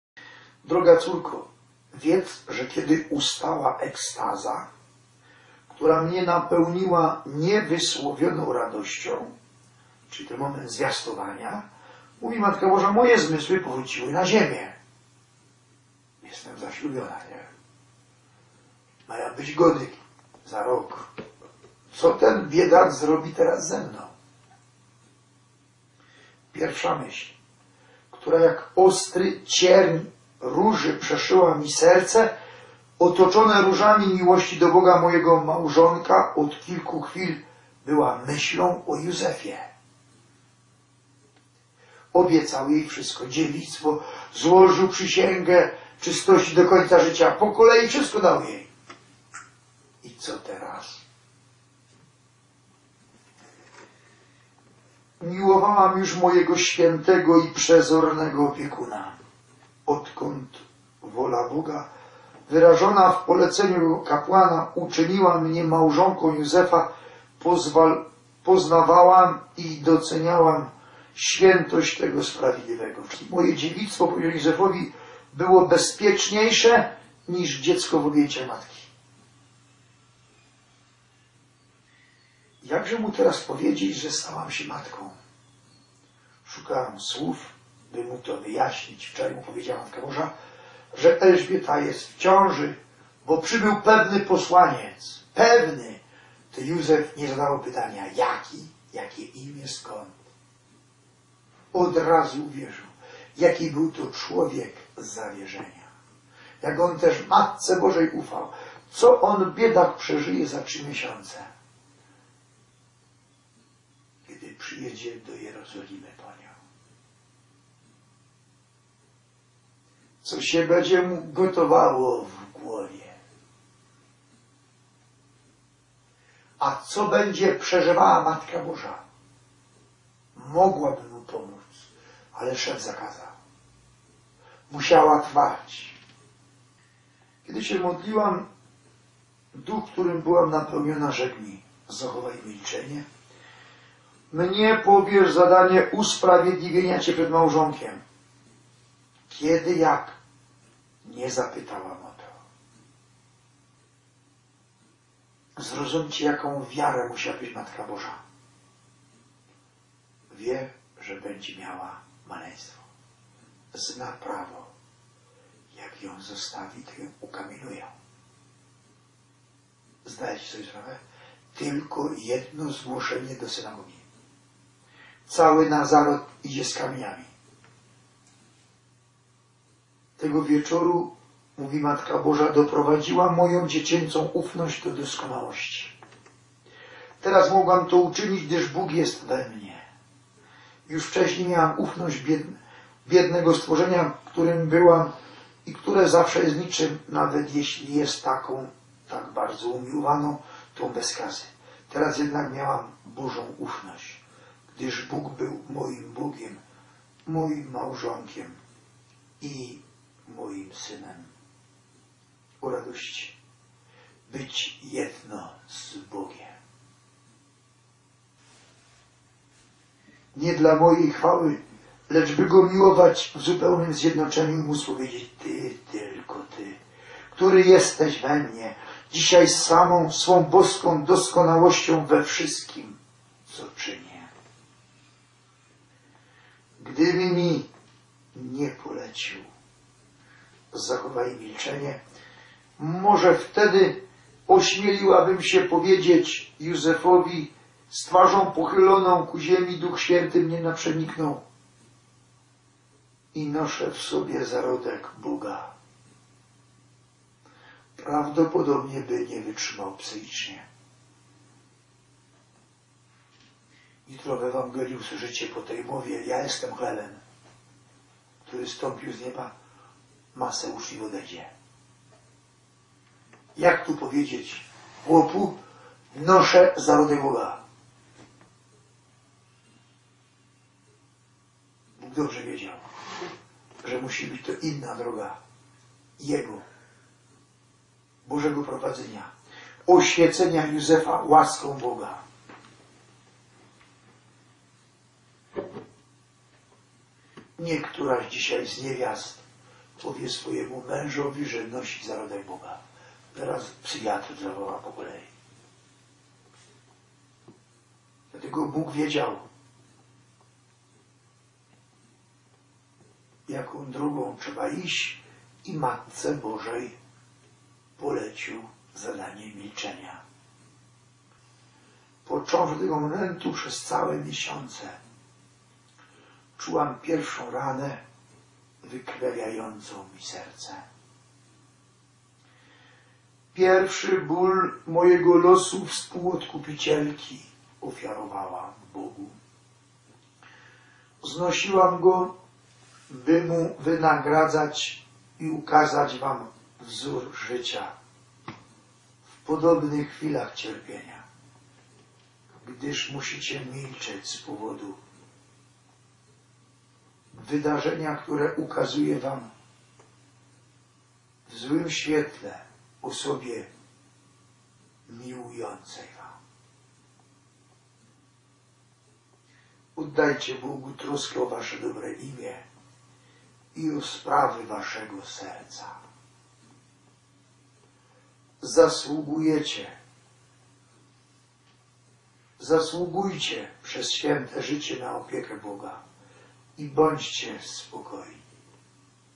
Ks. Natanek. Maryja - jakże powiedzieć Józefowi, że zostałam Matką?
Fragmenty kazania z dnia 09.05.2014r. z cyklu 'Polsko, Ciebie tylko Maryja uratuje" cz.184